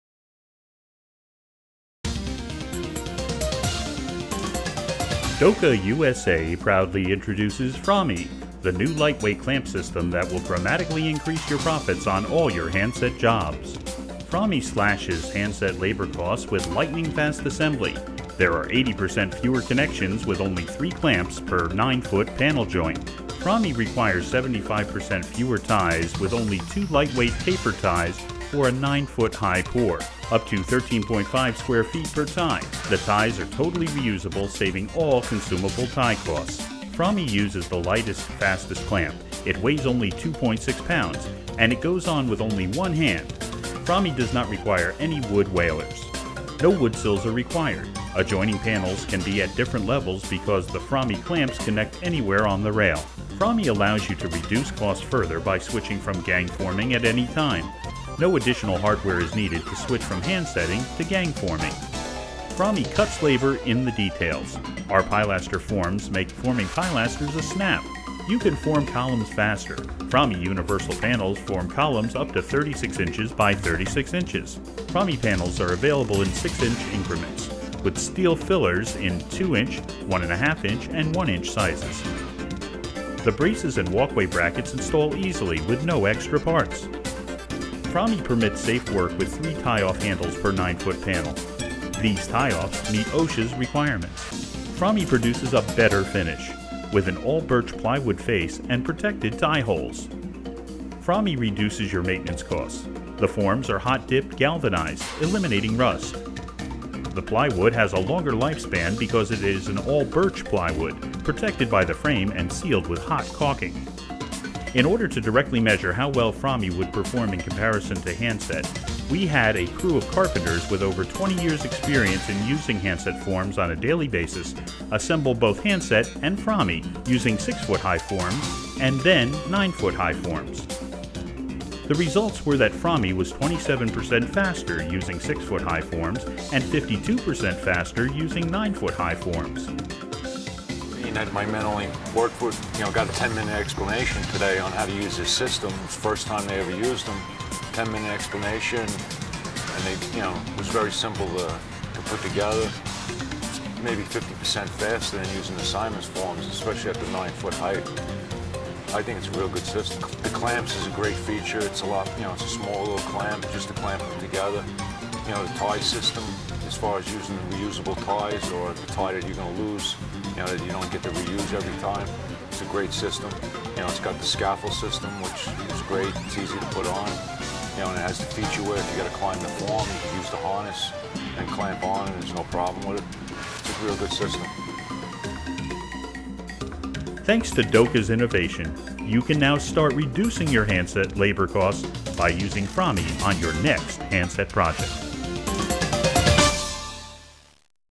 Frami Framed Formwork
Doka's Frami Framed Formwork
A lightweight clamp system in feet and inches

Frami is a lightweight clamp system for fast, economical forming of foundations and small areas.  Manufactured for the U.S. market, the measurements are in feet and inches.  One panel can be used to form a 9-foot wall, and with just two ties the panel can withstand full liquid head.  The ingenious modular design (with six different widths and three different heights of panel) makes for optimum adaptability to all job site conditions.  Doka's Frami system gives you unlimited possible combinations, in both width and height. The panels can be used either upright or horizontal, and the 6'' increment together with the steel fillers (2", 1 ½ ", 1") gives you optimum adaptability of the formwork to the dimensions of the structure, at all times.  Available for hand-set or ganged applications, the panels can be used for walls, columns, grade beams, and footings.  Designed for perfect inter-compatibility, the Doka framed formwork systems let you form any wall both quickly and easily. No matter how high or long the wall, or what the layout is like, you can depend on the flexible and easily adjustable Doka framed formworks to speed up the construction sequence.